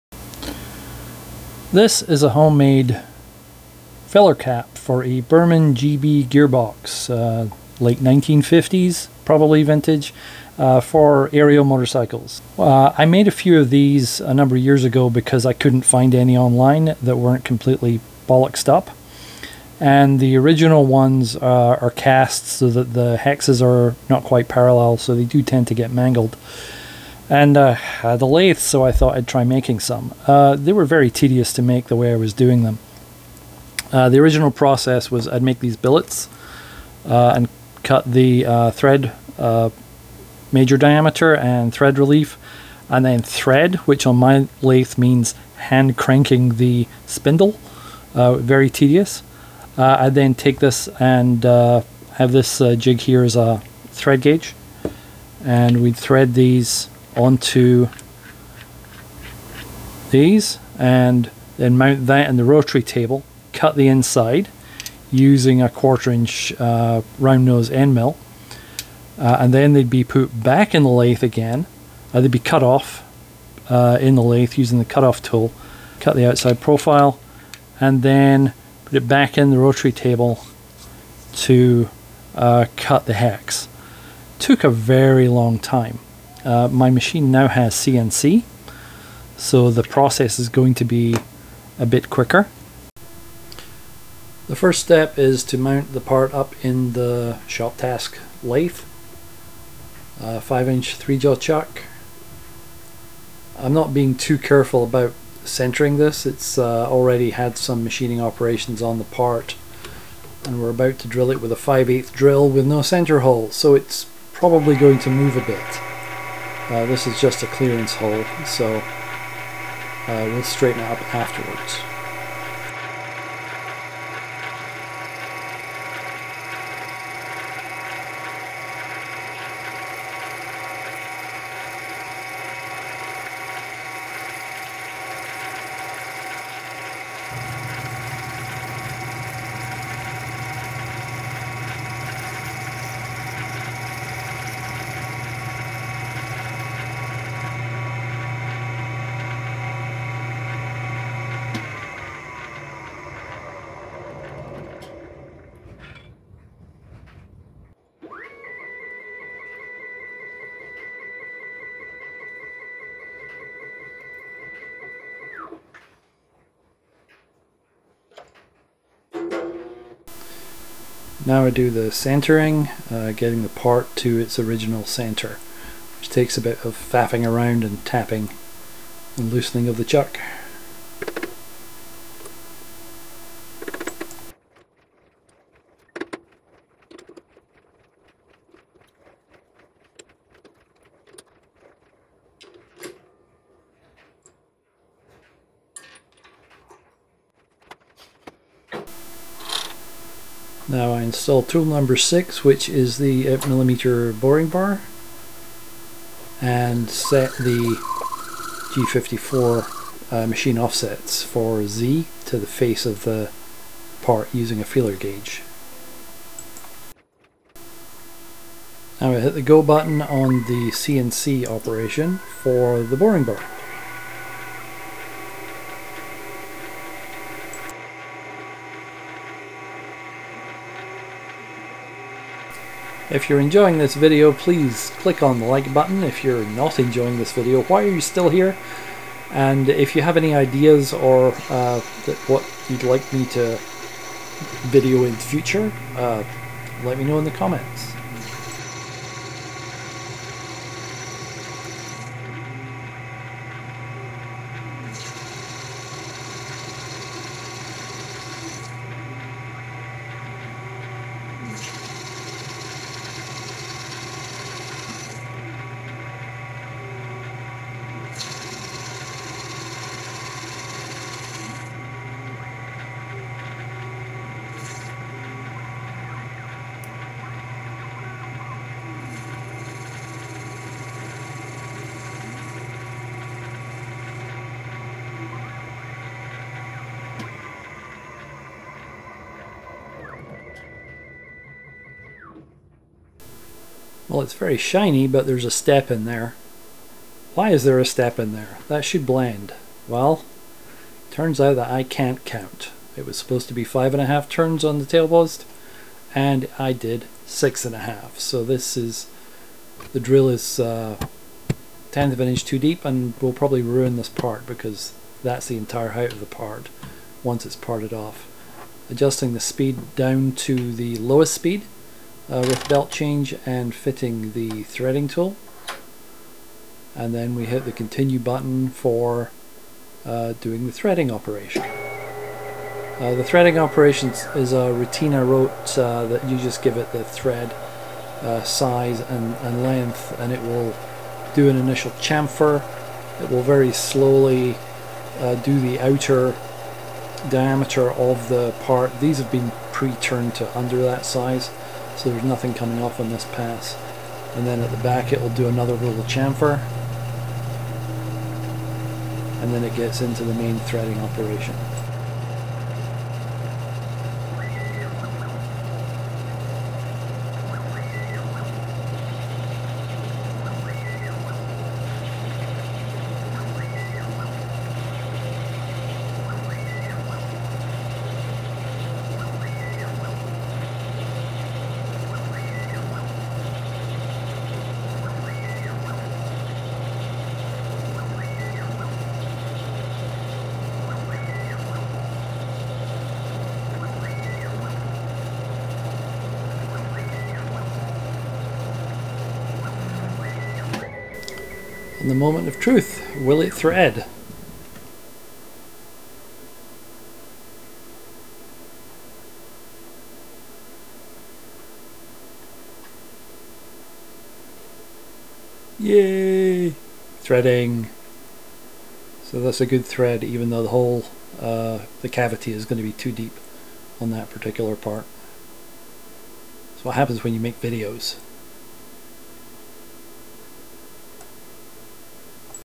Machining a Burman Oil Filler Cap on a LinuxCNC Metal Lathe - Part 1 - Boring and Threading
I tried to make these parts a long time ago when they were not easily available and I did not have CNC. I completed a few but the process was so time consuming that I abandoned the project and I have had a drawer full of half-made billets ever since. Now that I have LinuxCNC on my ShopTask Lathe Mill Combo, I'm completing these as a learning exercise.

The part in question is Ariel Motorcycle part# 3341-52 , which is the oil filler cap for the Burman GB gearbox fitted to all models 350cc-1000c from 1952 to 1959, including the Red Hunter, Fieldmaster, Huntmaster and Square Four (Squariel).

The material used is Fortal offcuts purchased on eBay - this is some kind of 7000 series aluminum alloy.

The design has a 3/4" hex which is larger than the originals, which were cast with a draft angle and no spanner ever fit right.

This episode covers the introduction and machining the interior cavity and thread (1-1/4" x 20TPI).

I am a hobbyist and maker and sometimes I sell what I make.